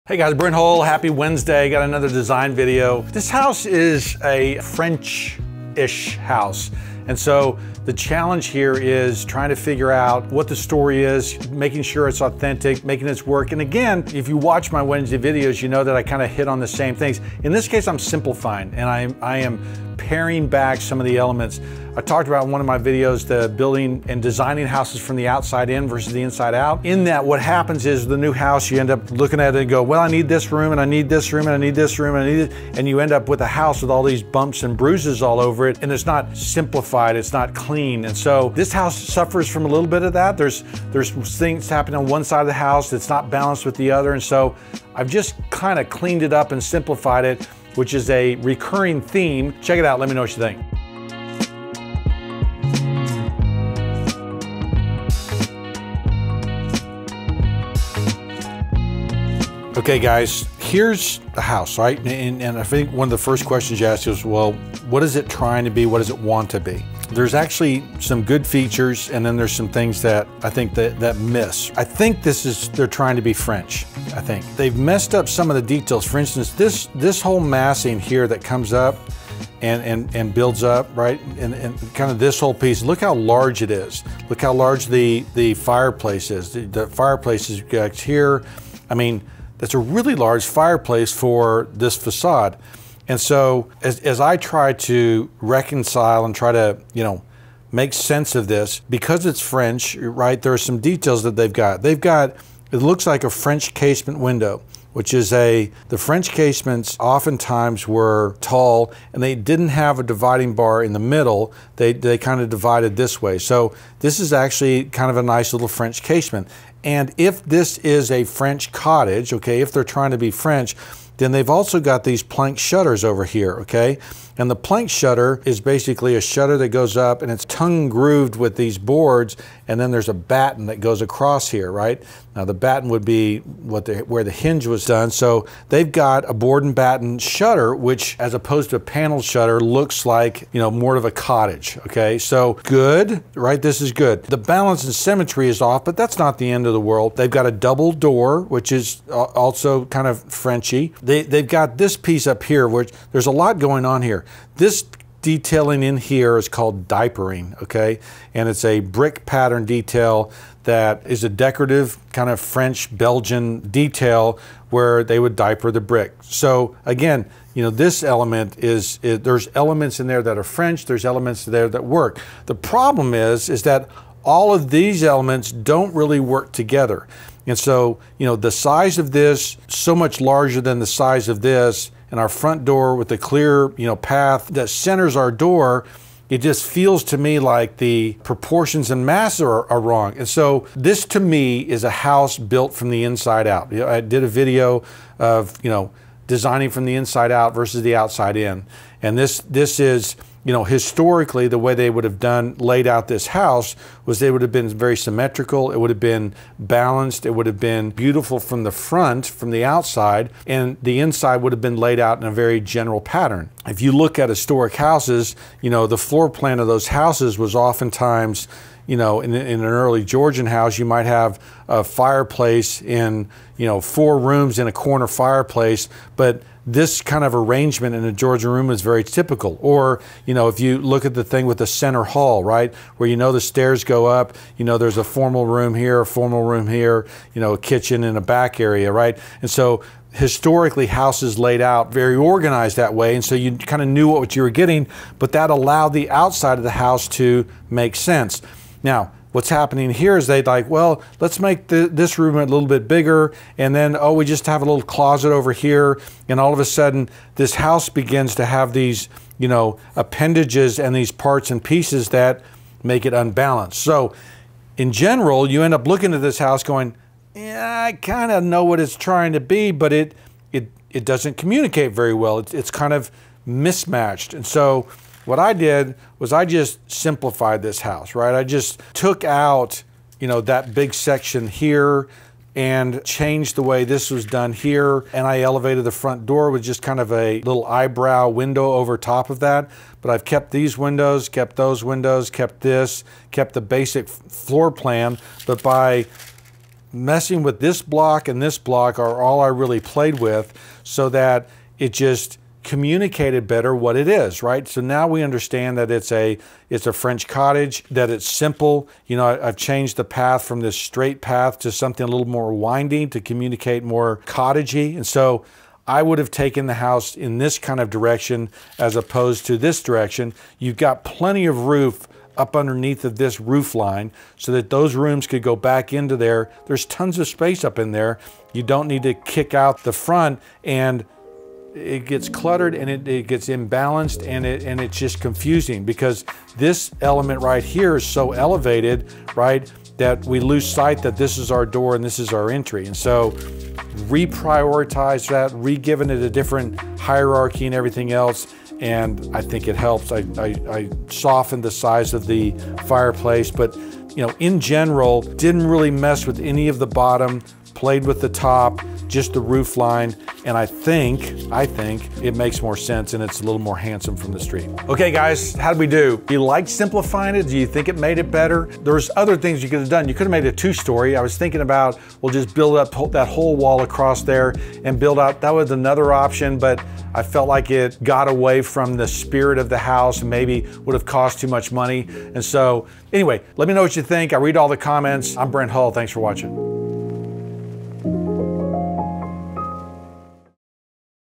French-ish house fix.
Brent helps a house that is trying to be French by simplifying details. Come check it out.

Here are a collection of books used in this talk in my Kit.Co library

Design book for houses 1920- Architect Small House plan book
500 Small houses of the 20's- Good designs for period revival homes
Samuel Chamberlain's drawings of Rural France